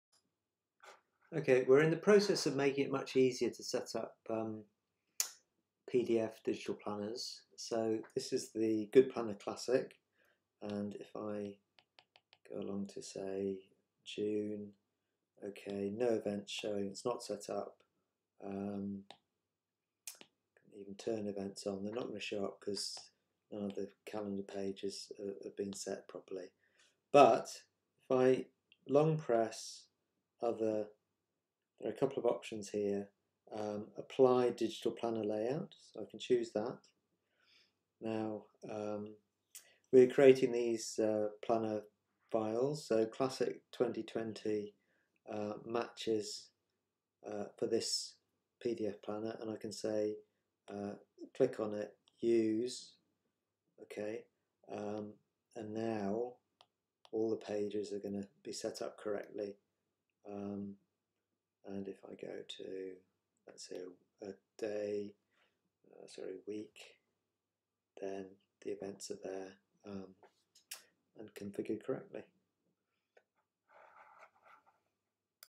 Planner Config files to aid Pdf planner setup
A planner config file may be available for particular pdf planners - if it is then it makes setting up a pdf planner for events much easier.